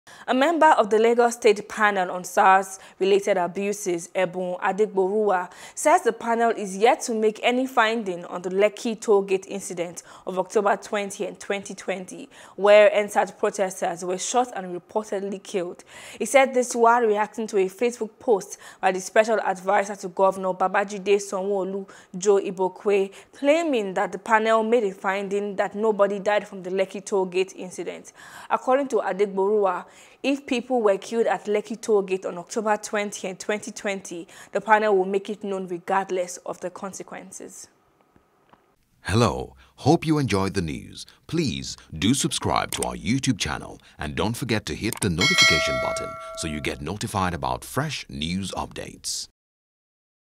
Panel Won’t Cover Up Killings At Lekki Tollgate - Adegboruwa | NEWS
A member of the Lagos State Panel on SARS Related Abuses, Ebun Adegboruwa says the panel is yet to make any finding on the Lekki tollgate incident of October 20, 2020, where EndSARS protesters were shot and reportedly killed.